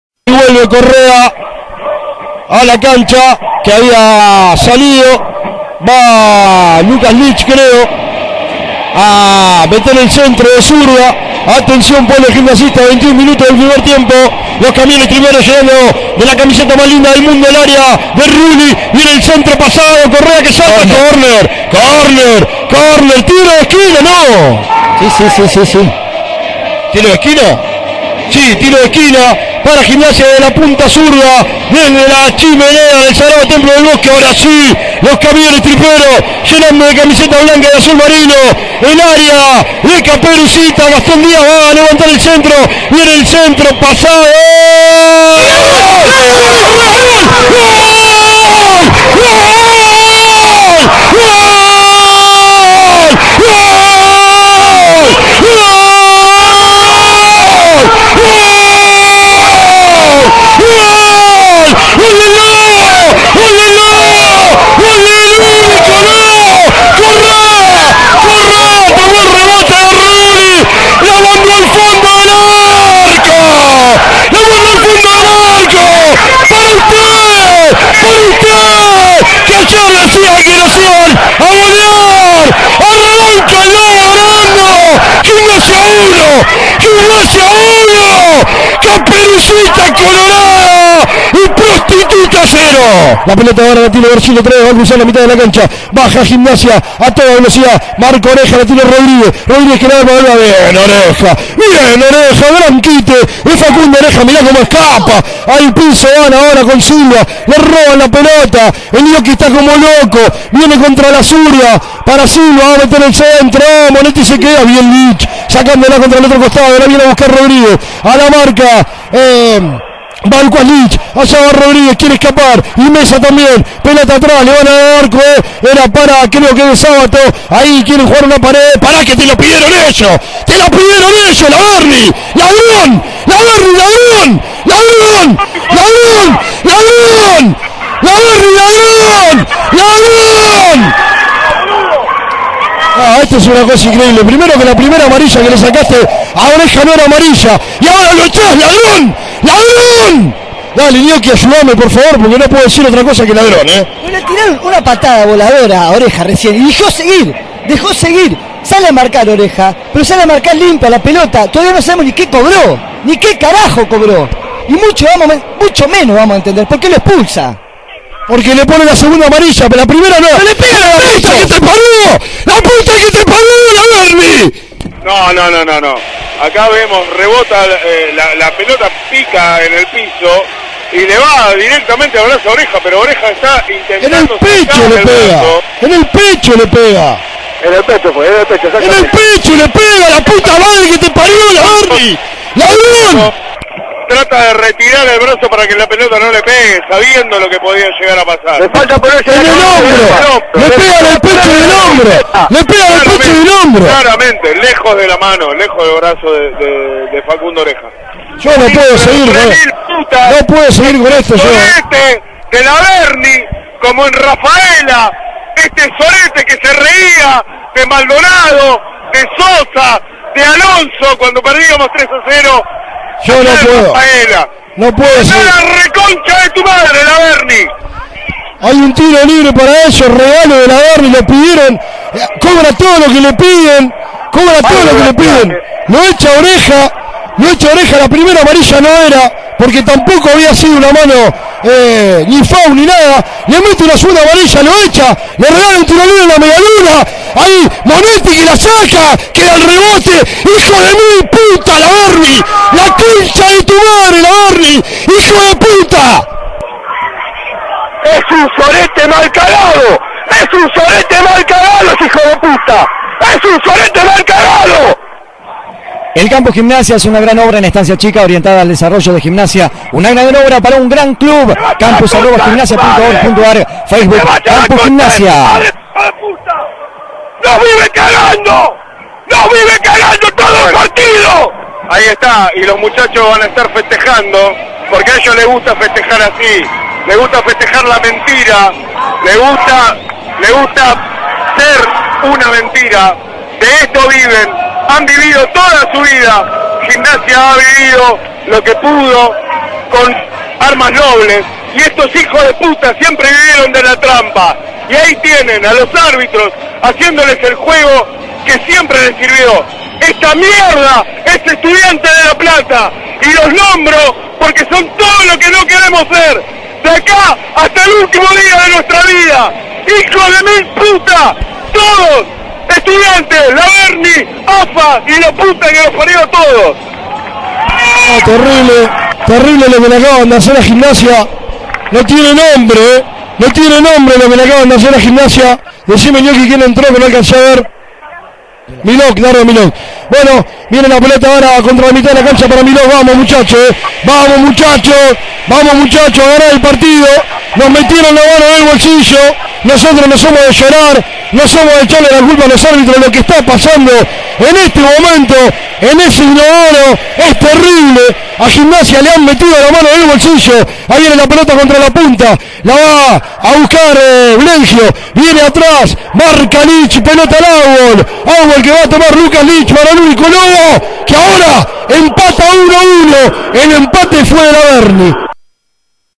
Estudiantes 1 Gimnasia Lp 1 - El relato de Alberto Raimundi NO TIENE DESPERDICIO!!!
Relator: Alberto Raimundi

"El campo Gimnasia es una gran obra en Estancia Chica, orientada al desarrollo de Gimnasia, una gran obra para un gran clubTE VAS A LA CONCHA DE TU MADREEEE, TE VAS A LA CONCHA DE TU MADRE, ESE HIJO DE PUTA NOS VIVE CAGANDO, NOS VIVE CAGANDO TODO EL PARTIDO!"

Aclaración: No soy ni de Estudiantes ni de GELP, pero el relato me pareció genial.